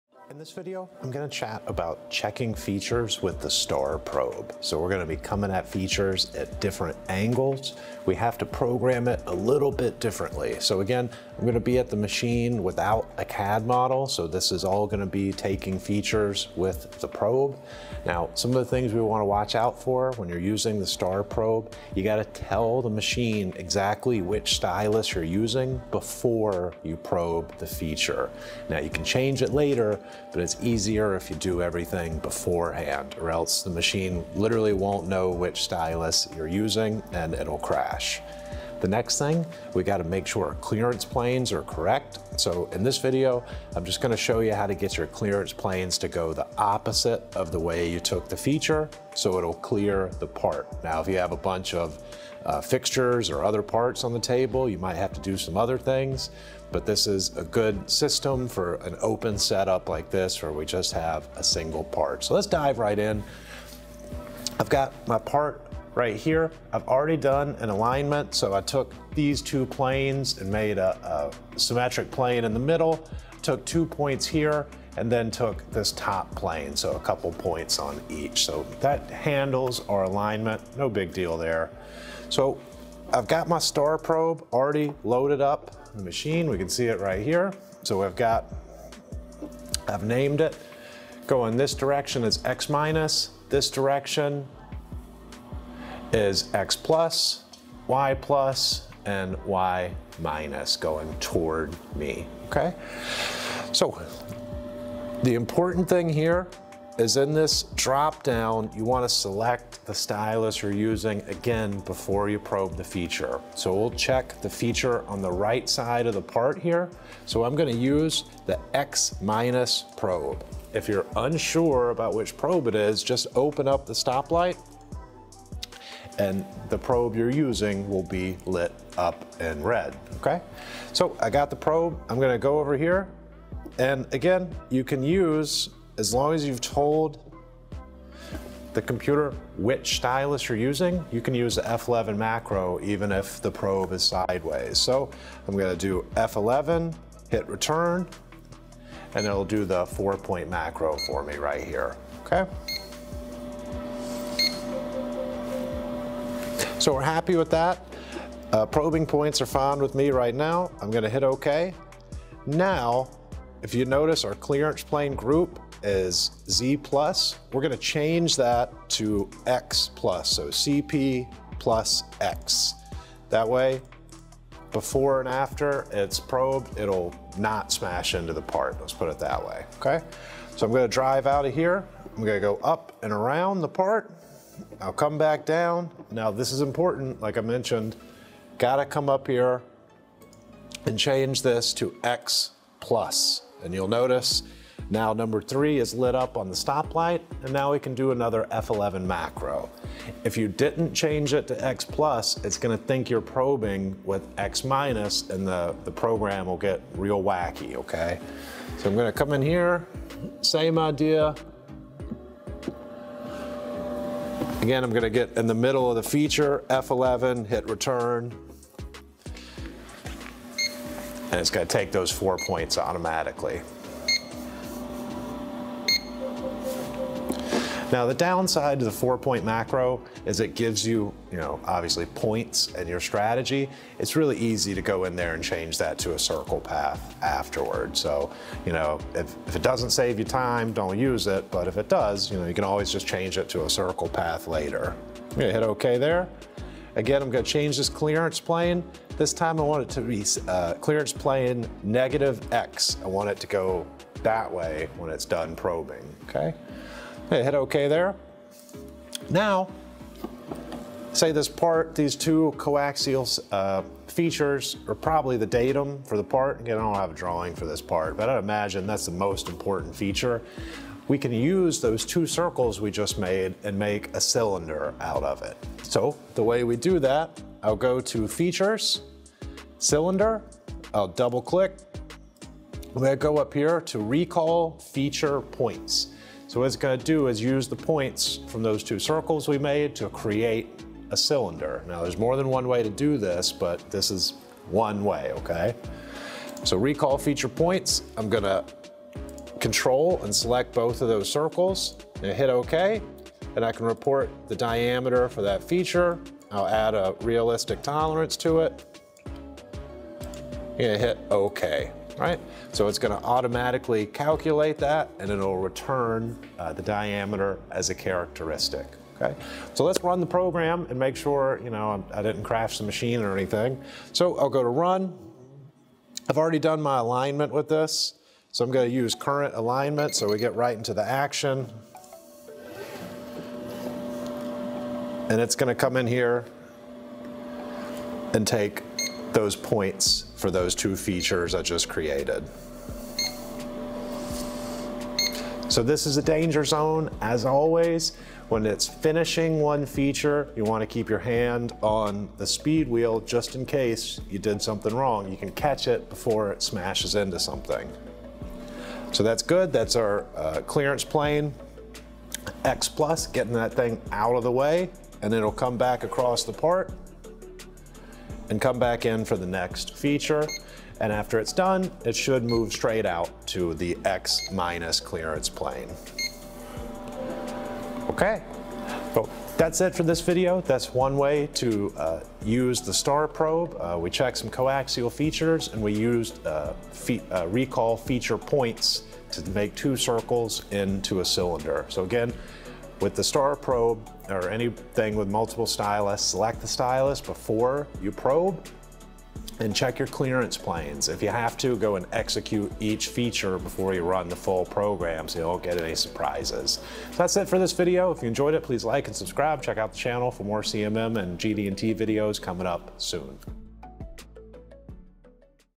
Zeiss CMM: Star Probe Programming - No CAD Model
I show how to write a program with multiple styli, without a CAD model.